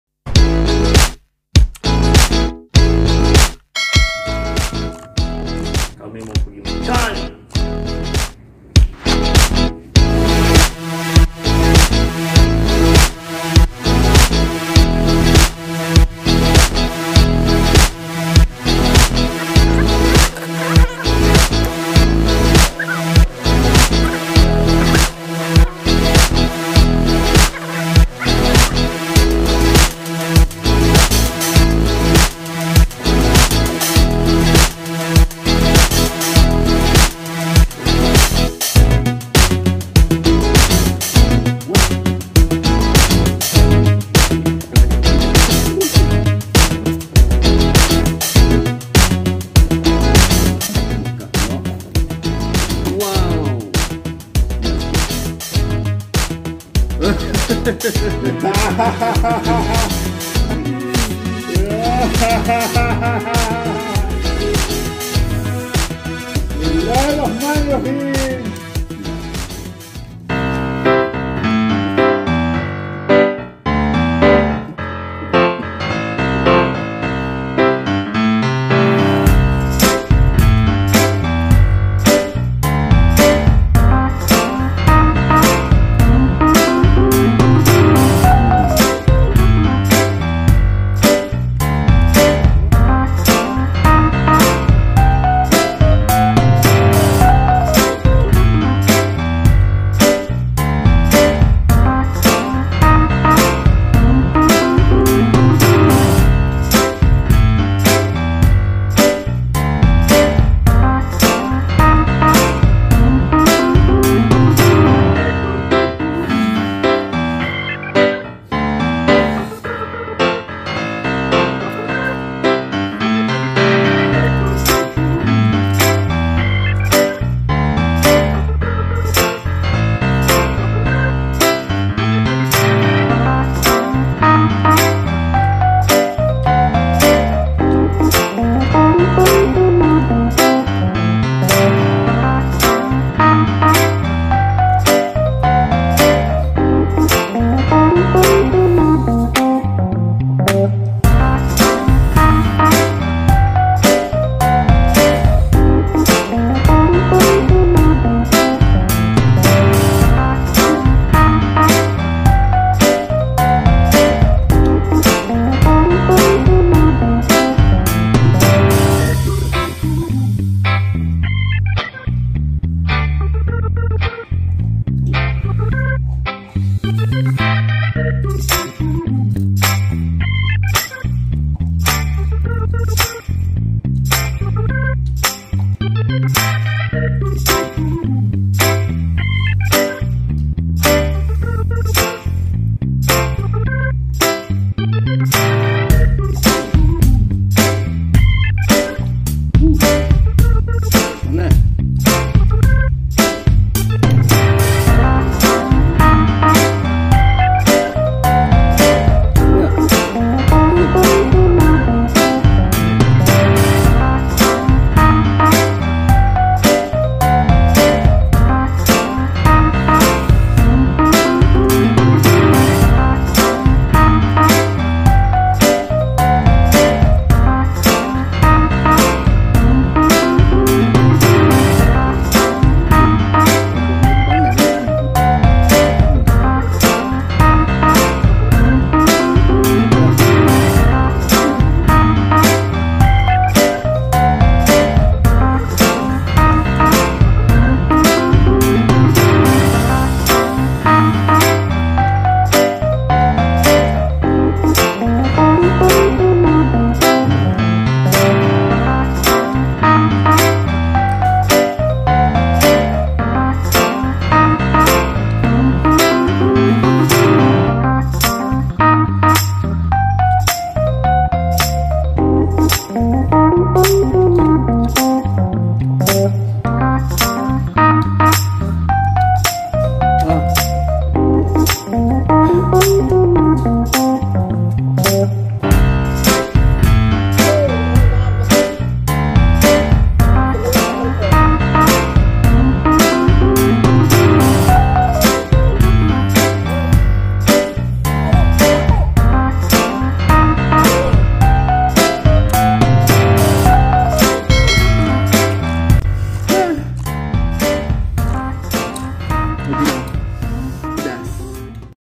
Lapar Makan Lepak
Lapar.. Layan meggi pedas sampai berasap